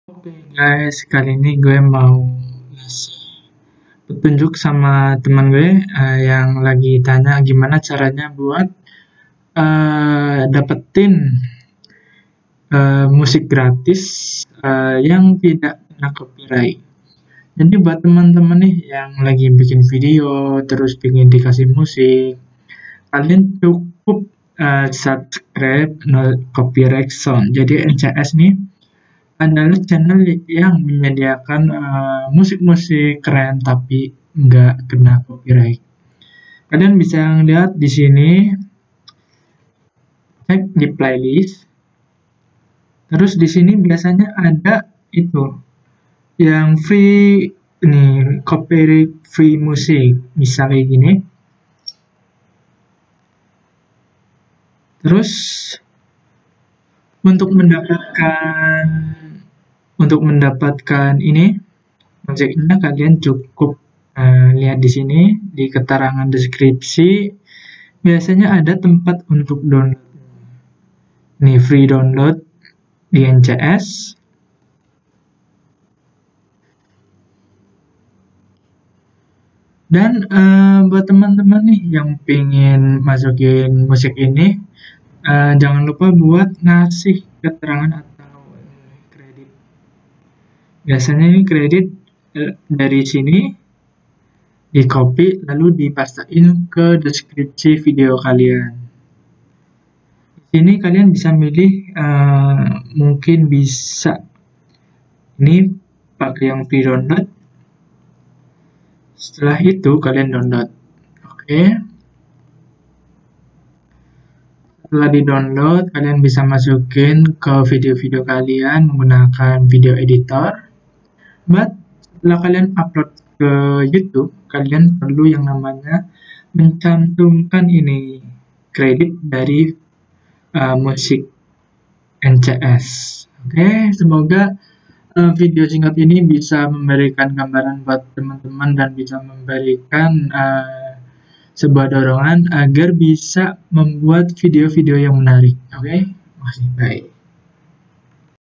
Cara Download Music Gratis di NCS atau NoCopyRightSounds tanpa kena Copyright
Jangan Lupa gunakan Kualitas HD untuk melihat videonya agar code dapat dilihat dengan jelas.

Untuk download music gratis No Copyright di NCS, kalian bisa ikuti langkah berikut :
1. Buka Channel NoCopyrightSounds atau NCS
3. Pilih Copyright Free Music For Youtube.
4. Download music nya.
5. Masukan Credit di deskripsi video Youtube Kalian